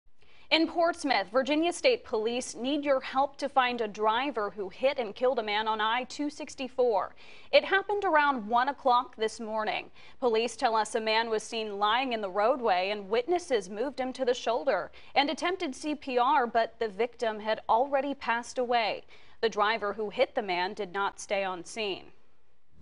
Man dies in hit-and-run on I-264 in Portsmouth
Police responded to the call just before 1 a.m. for the report of a crash involving a pedestrian westbound on interstate 264 at exit 6/MLK Expressway.